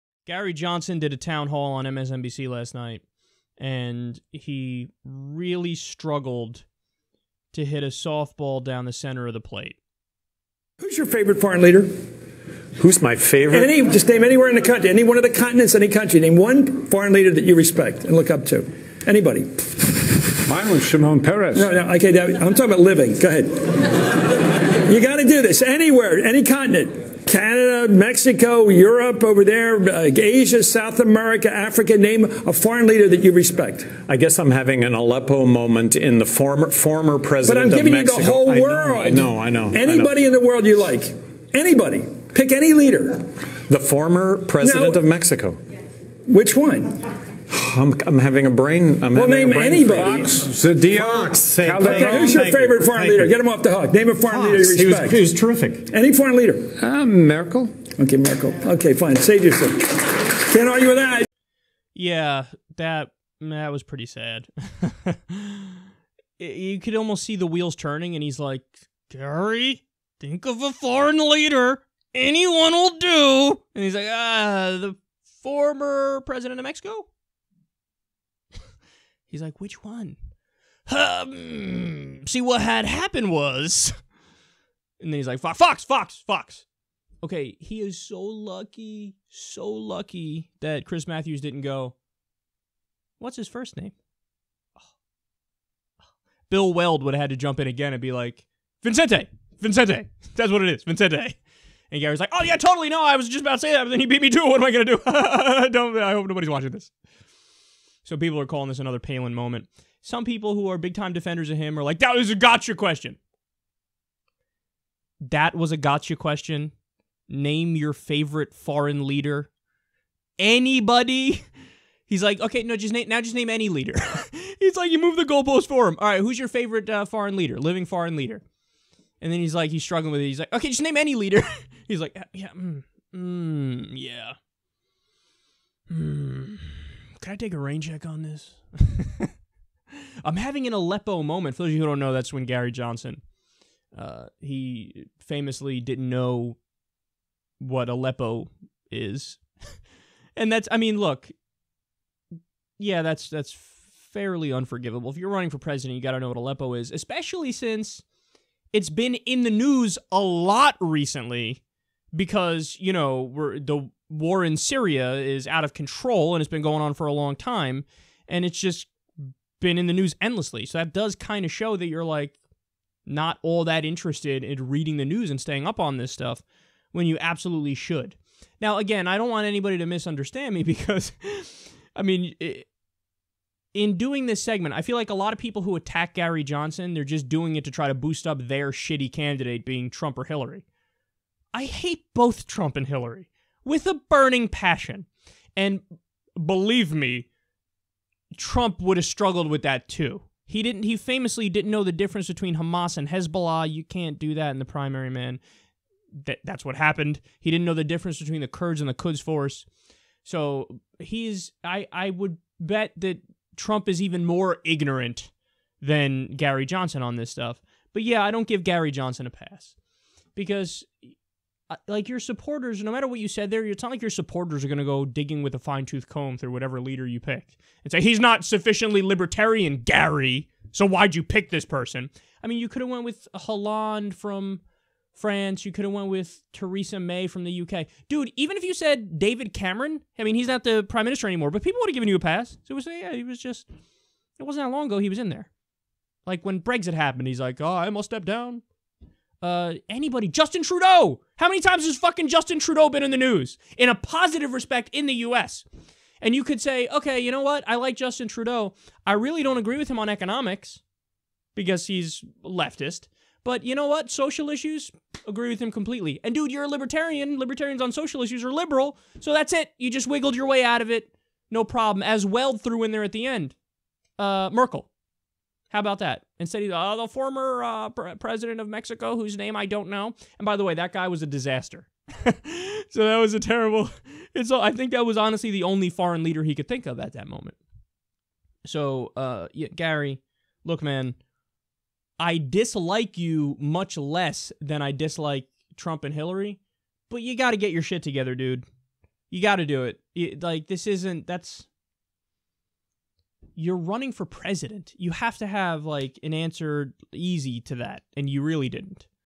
Gary Johson Faceplants Trying To Name Favorite World Leader
Asked to name his favorite foreign leader, or any foreign leader he admires, Libertarian nominee for president Gary Johnson was unable to come up with an answer...